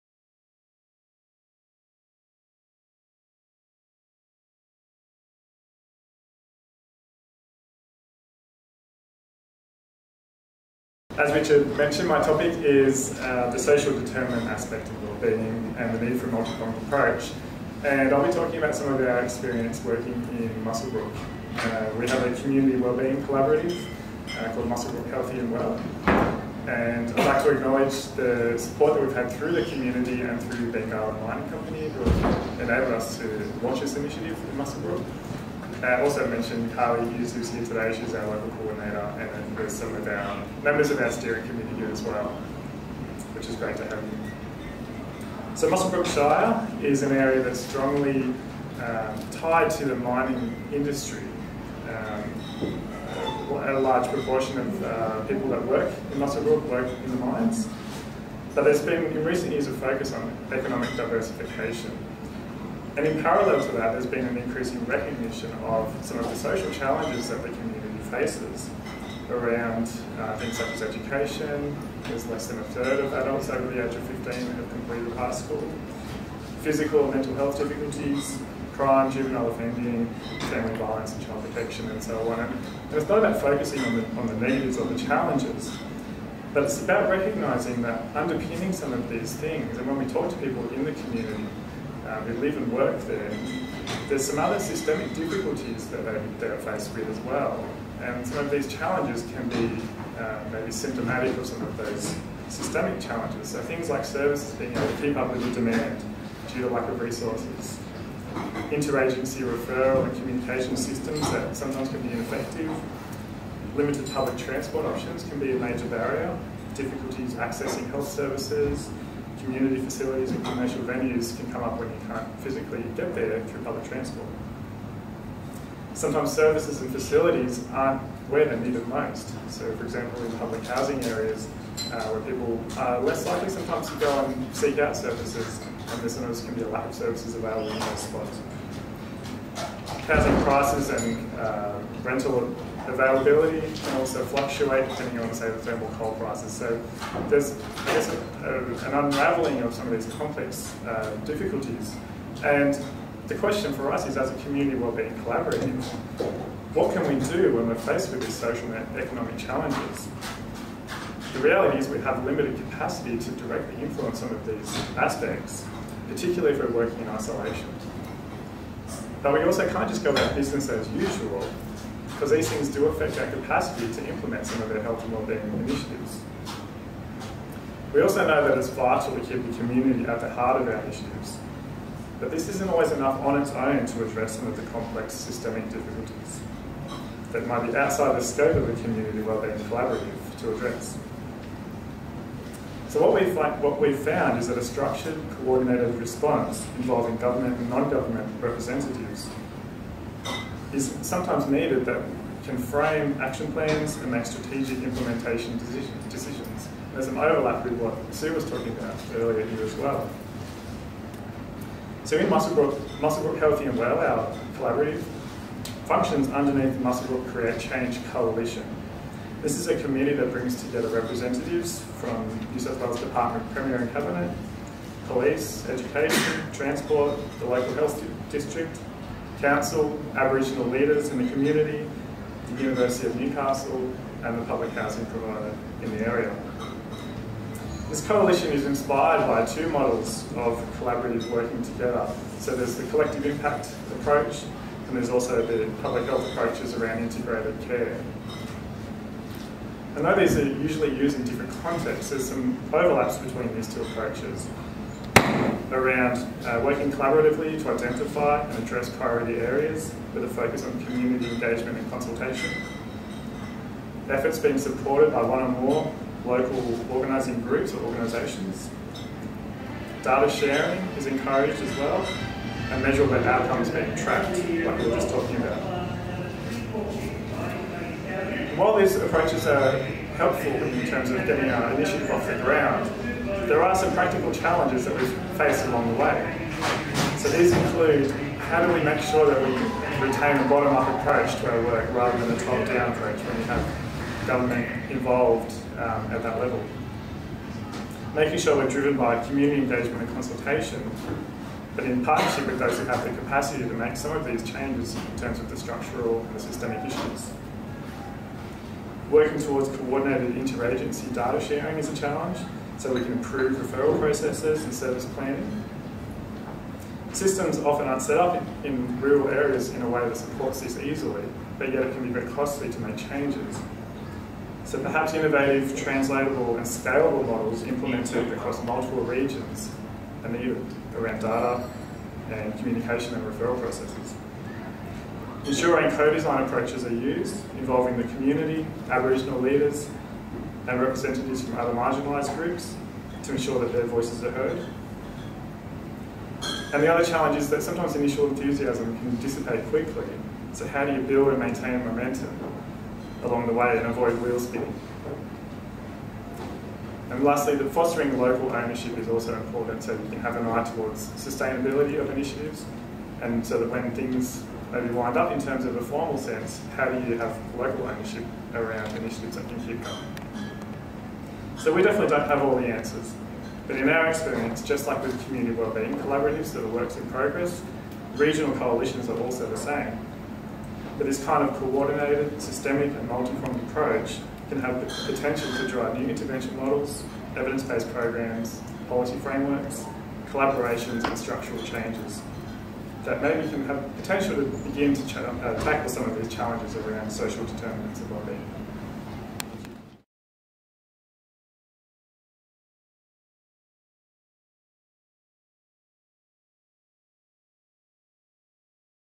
The social determinant aspect of community wellbeing | Dr Jamin Day
Dr Jamin Day, Postdoctoral Research Fellow, Family Action Centre, University of Newcastle, speaks at CRRMH's 2019 Rural Suicide Prevention Forum.

The 2019 Rural Suicide Prevention Forum discusses community wellbeing collaboratives and the need for a coordinated approach to wellbeing and suicide prevention initiatives in rural communities.